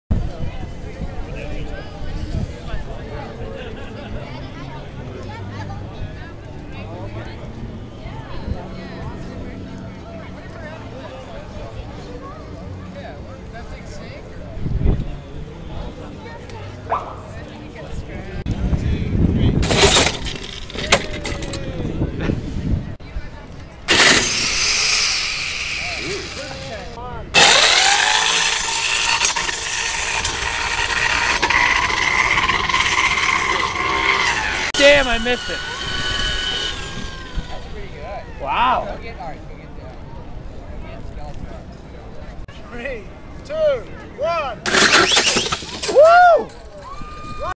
Power Tool Drag Racing Test Runs and Crashes
At Maker Faire 2008, we catch some exclusive video of power tool drag racing.